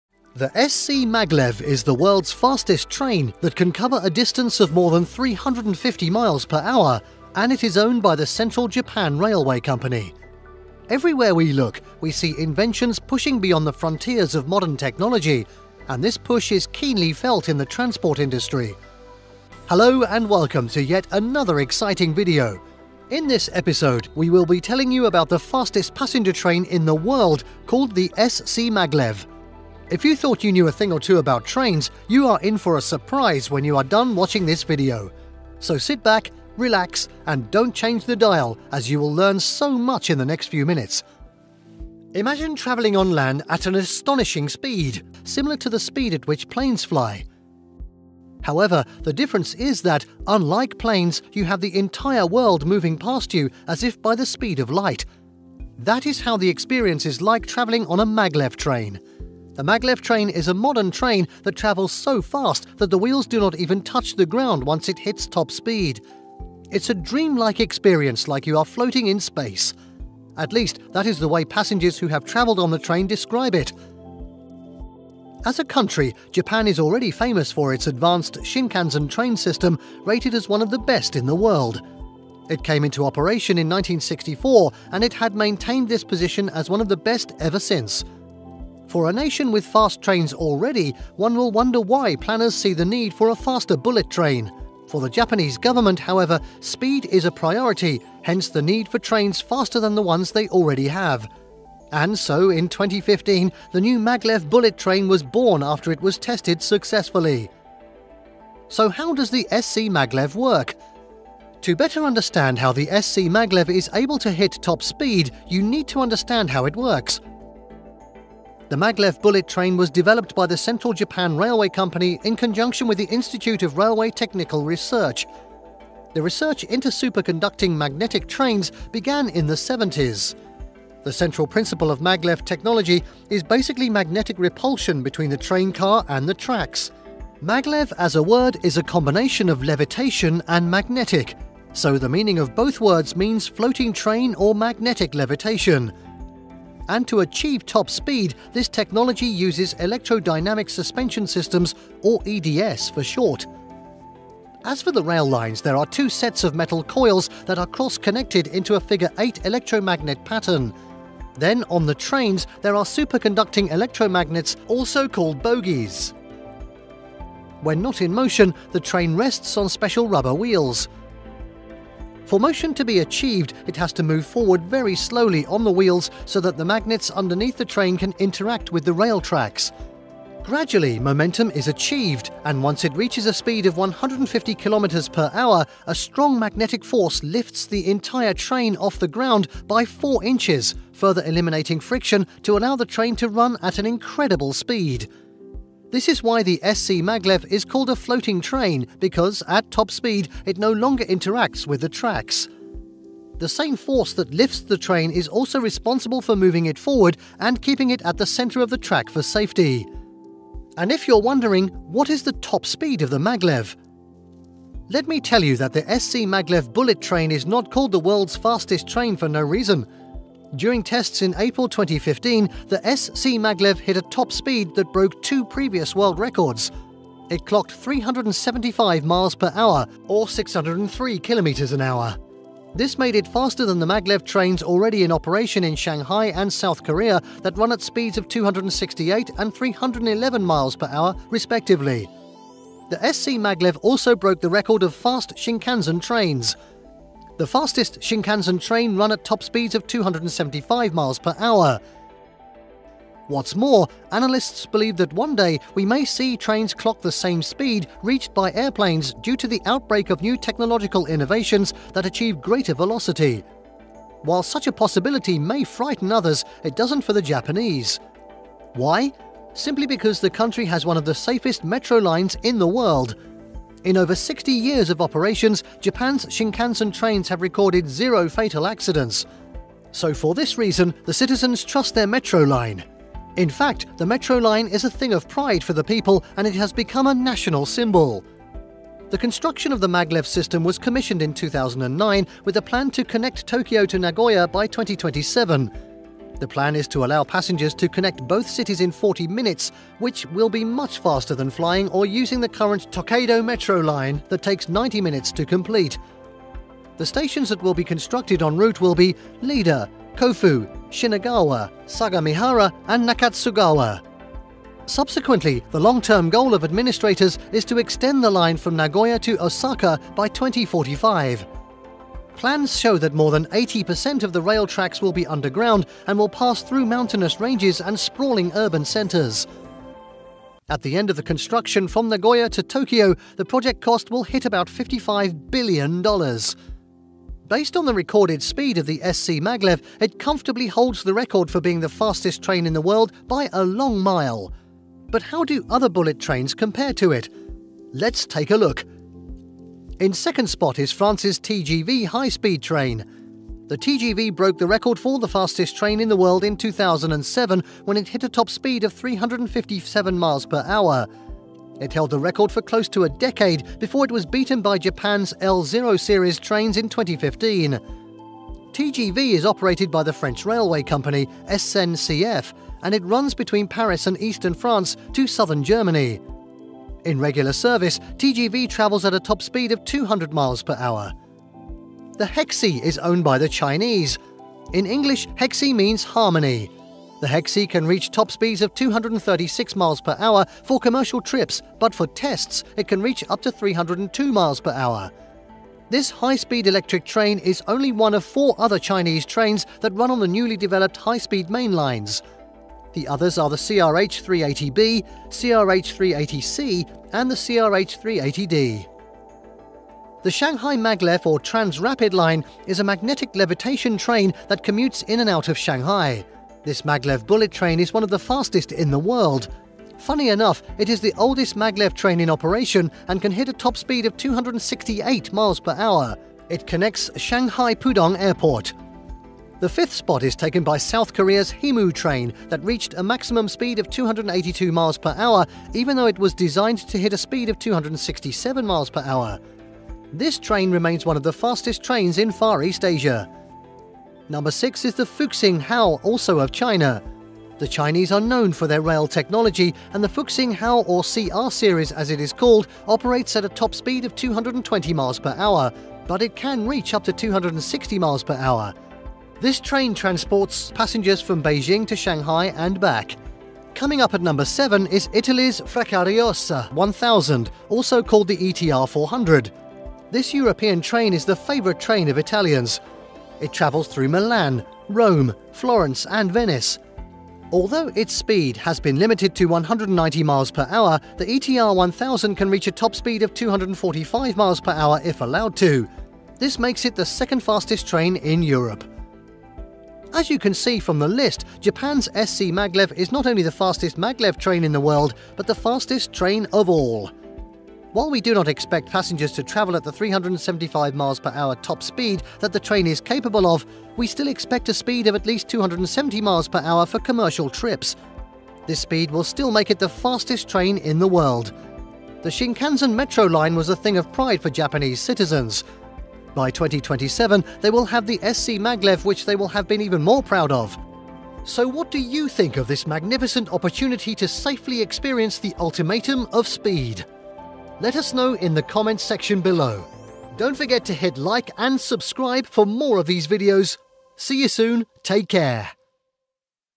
The SCMaglev - The World's Fastest Train
The SCMaglev is the world’s fastest train that can cover a distance of more than 350 miles per hour and it is owned by the central Japan railway company. Everywhere we look, we see inventions pushing beyond the frontiers of modern technology, and this push is keenly felt in the transport industry. Hello and welcome to yet another exciting video. In this episode, we will be telling you about the fastest passenger train in the world called the SCMaglev. If you thought you knew a thing or two about trains, you are in for a surprise when you are done watching this video.

So sit back, relax, and don’t change the dial as you will learn so much in the next few minutes.